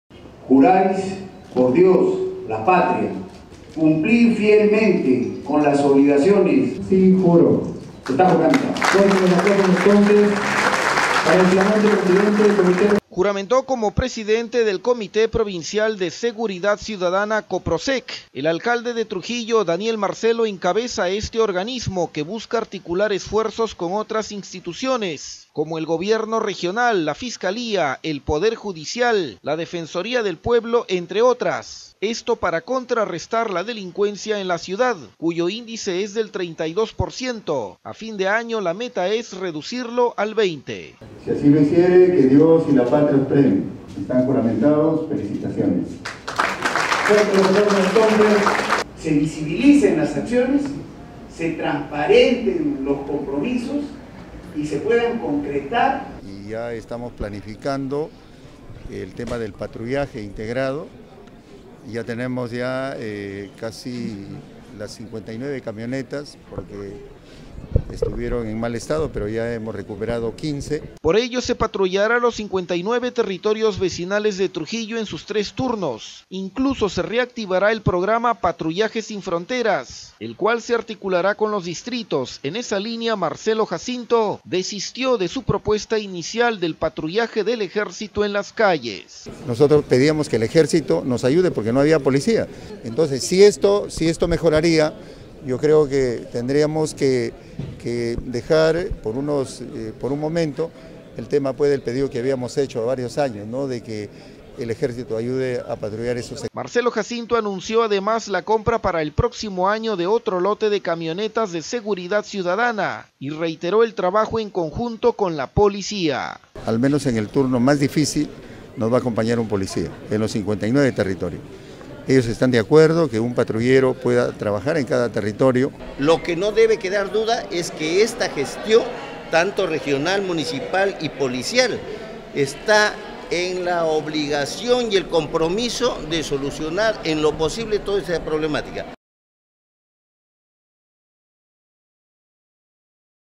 Trujillo: Coprosec juramenta y reactivarán "Patrullas sin frontera"
Daniel Marcelo desistiría de su propuesta inicial del patrullaje del Ejército en las calles.

El alcalde de Trujillo, Daniel Marcelo, juramentó como presidente del Coprosec y anunció la reactivación del programa “Patrullaje sin fronteras”.